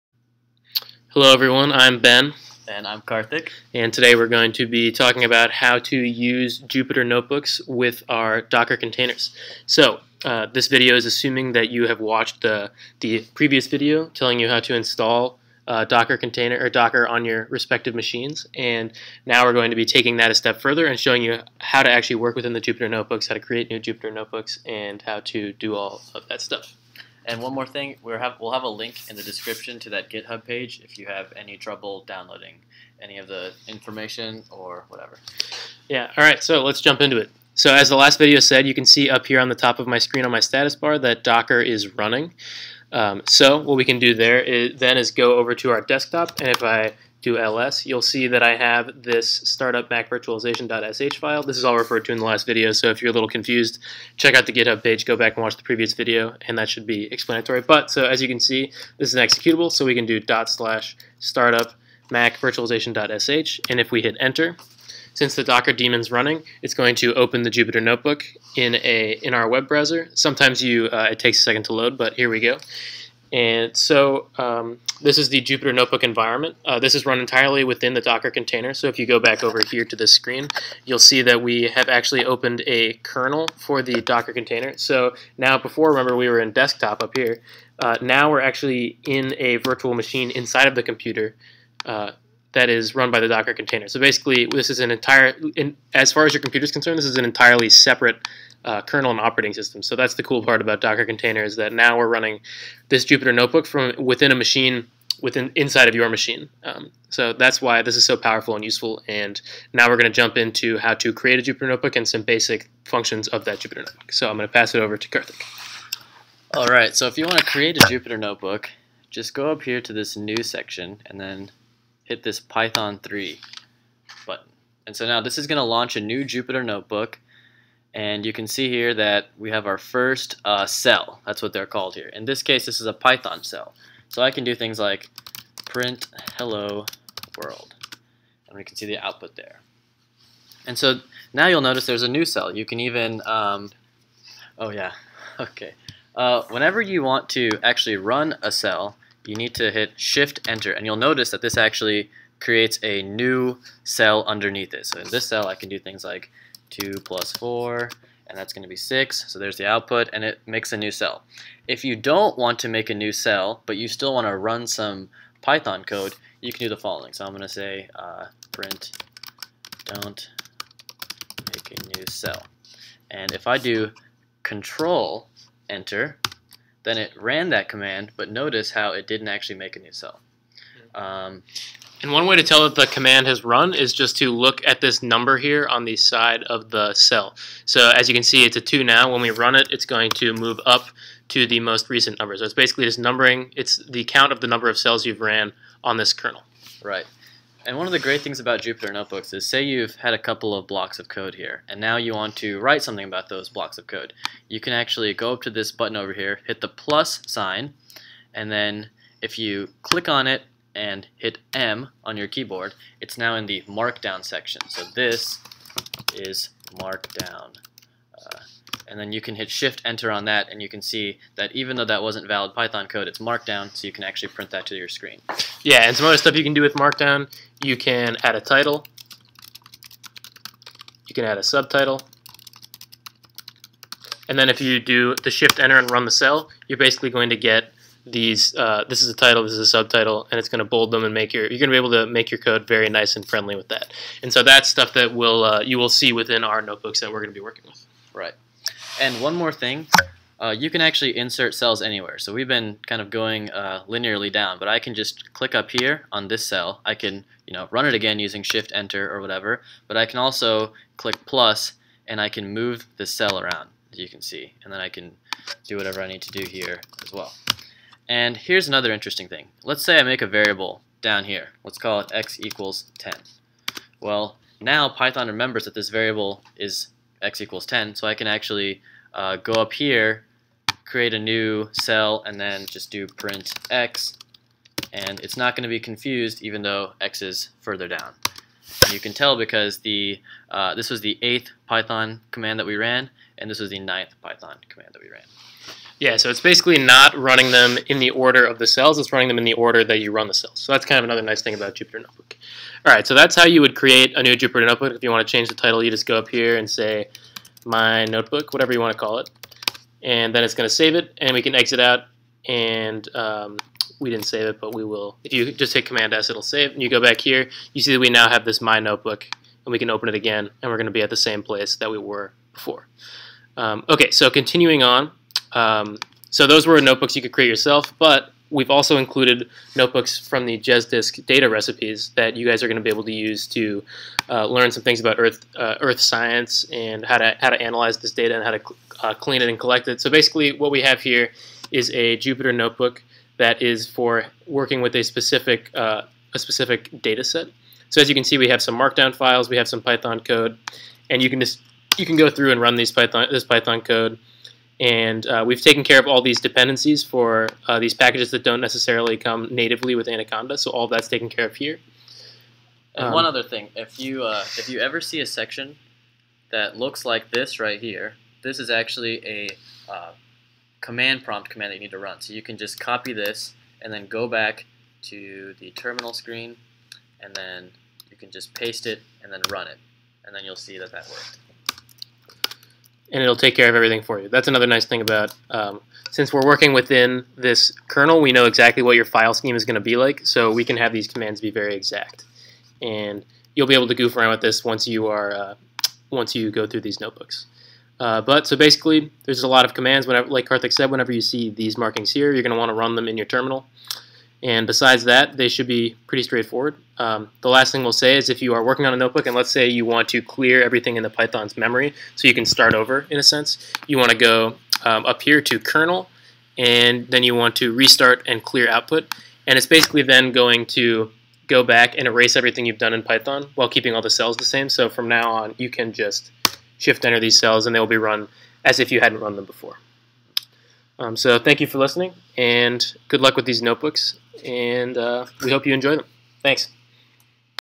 Jupyter Notebook Docker Tutorial
Tutorial for how to use jupyter notebook with docker. For information on how to install Docker for your machine, refer to our github page: